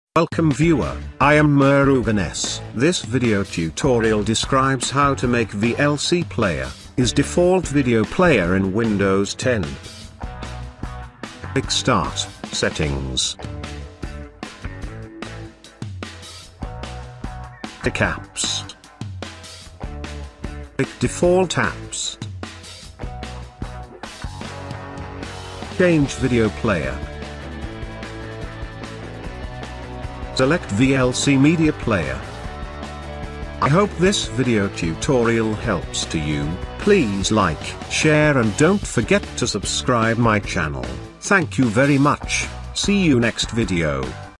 How To Make VLC Player is Default Video Player In Windows 10
This Tutorial helps to How To Make VLC Player is  Default Video Player In Windows 10